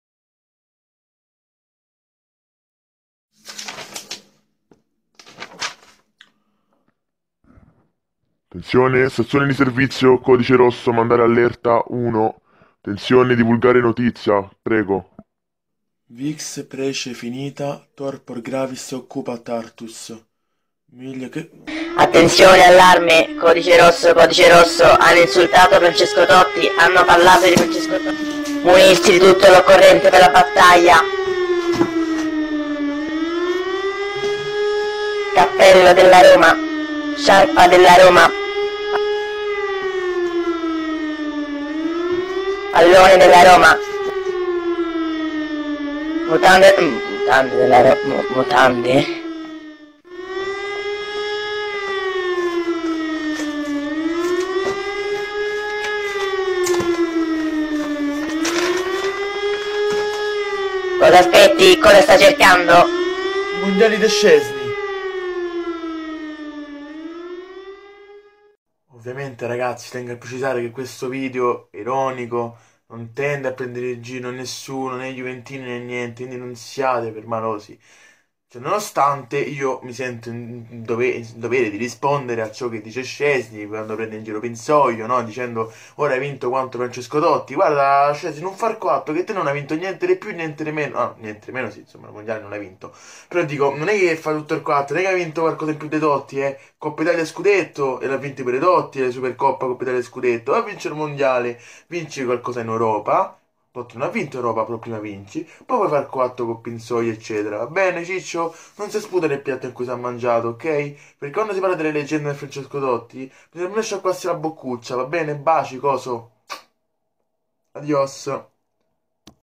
"TOTTI HA VINTO QUANTO PINSOGLIO" - SZCZESNY TACI, NON HAI VINTO NULLA!
LEGGETE PER FAVORE LA DESCRIZIONE !
VIDEO PARODIA E IRONICO, ARRIVATECE!
I numeri possono essere buoni per gli amanti delle statistiche, ma non sempre corrispondono al reale valore di un calciatore o all'impatto che ha avuto in una squadra. Un esempio chiaro? Basta prendere la battuta di Szczesny ai polacchi di Foot Truck, con il portiere della Juve che in qualche modo paragona Totti a Pinsoglio, attuale terzo portiere bianconero: "Pinsoglio è un bravo ragazzo - dice -, migliora l'atmosfera nello spogliatoio. Ha giocato 22 minuti lo scorso anno e ora lo prendo in giro dicendo che ha vinto lo stesso numero di titoli di Totti".

Rega passate da CALCIOBASSOCOSTO per maglie da calcio bellissime a prezzi davvero bassi!

ADIOOOOOOOOOOOOOOOOOOOOOOOOS